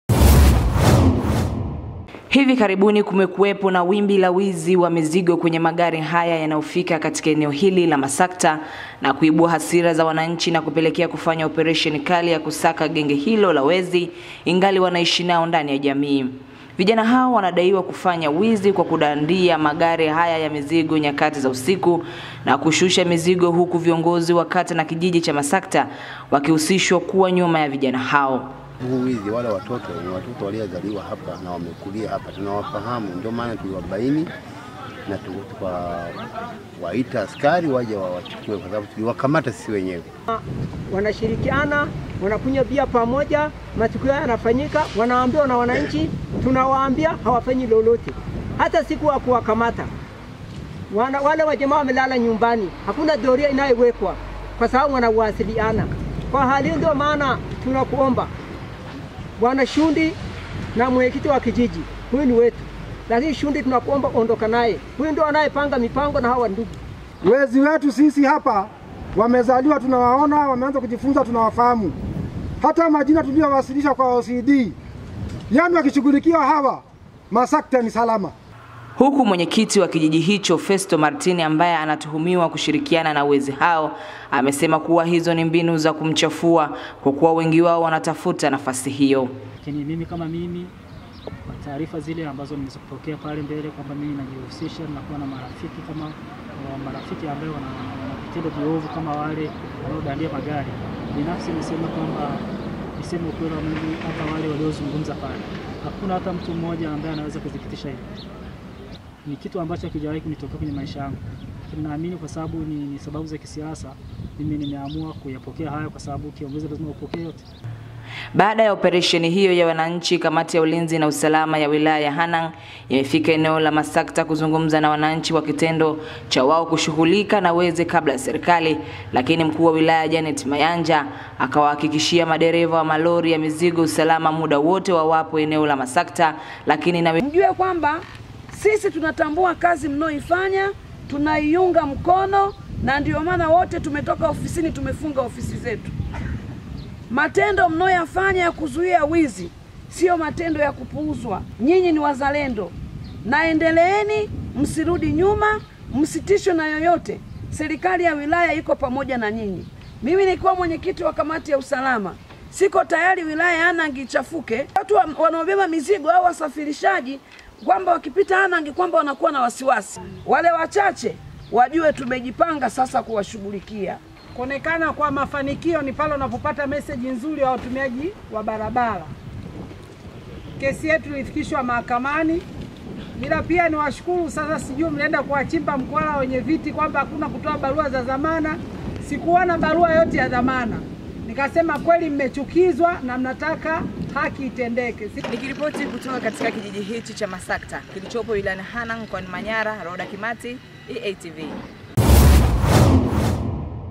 TAZAMA GENGE LA MAJAMBAZI LILILOKAMATWA NA WANANCHI BAADA YA KUWA TISHIO MANYARA
Wananchi wa kijiji cha masakta wilaya ya Hanang mkoani Manyara wamekamata Genge la wezi  vijana sita  na kuwakabidhi kwa jeshi la polisi huku wakiendelea na operesheni hiyo ya kuwasaka wengine wanaojihusisha na wizi huo.